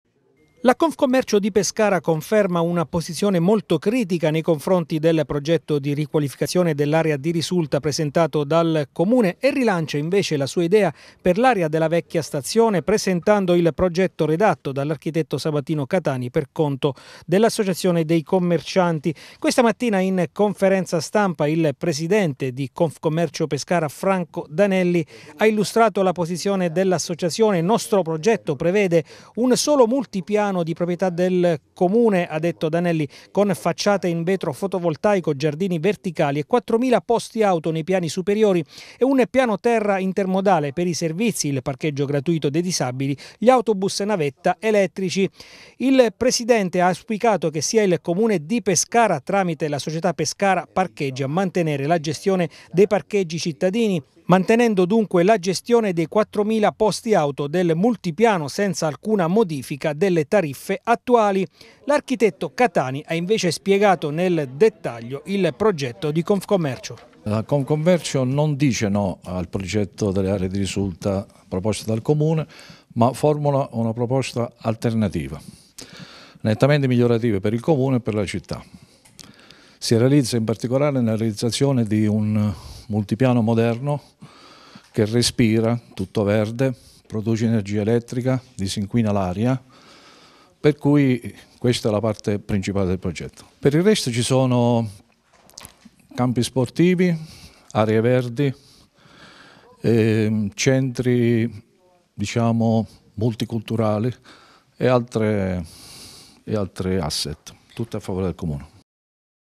Pescara. Confcommercio critica il progetto area di risulta
La Confcommercio di Pescara conferma la sua posizione critica nei confronti del progetto di riqualificazione dell’Area di Risulta presentato dal Comune, e rilancia la sua idea per l'area della vecchia stazione, presentando il progetto redatto dall’architetto Sabatino Catani